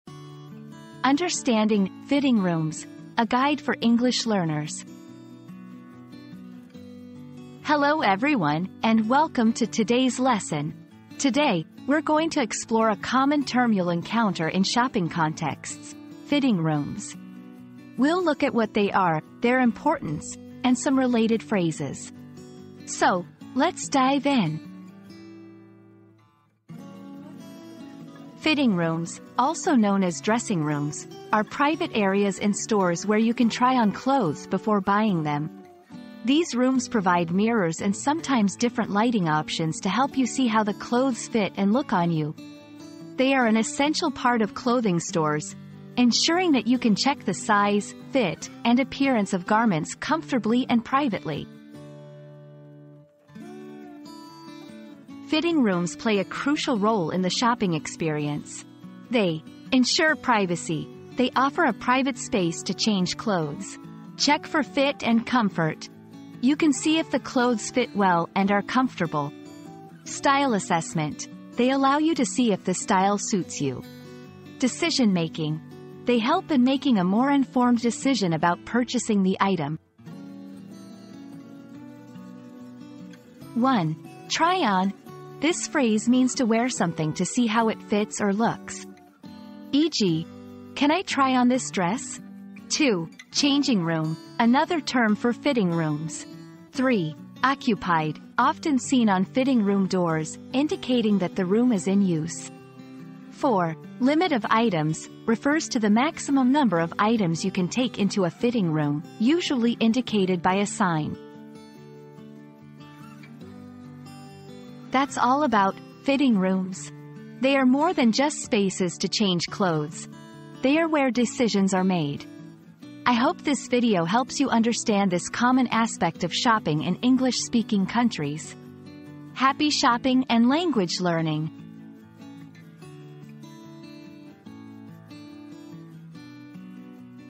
Understanding "Fitting Rooms": A Guide for English Learners
00:00 • Introduction - Understanding "Fitting Rooms": A Guide for English Learners
00:27 • What are "Fitting Rooms"?
00:54 • The Importance of Fitting Rooms
01:23 • Related Phrases and Usage